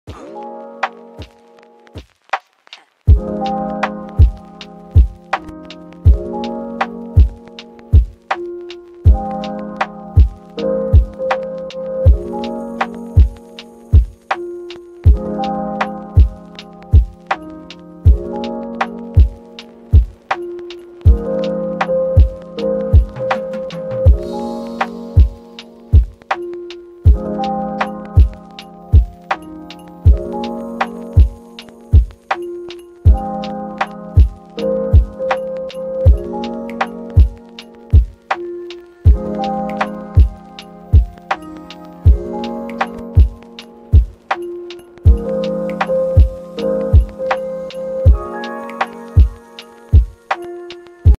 Fresh Impact Adidas for men
Fresh Impact by Adidas is a Aromatic Aquatic fragrance for men.

Fresh Impact was launched in 2009. The nose behind this fragrance is Harry Fremont.

Top notes are Watermelon, Amalfi Lemon and Green Notes;

middle notes are Ginger and Nutmeg;

base notes are Musk, Patchouli and Amber.

Musician: Rook1e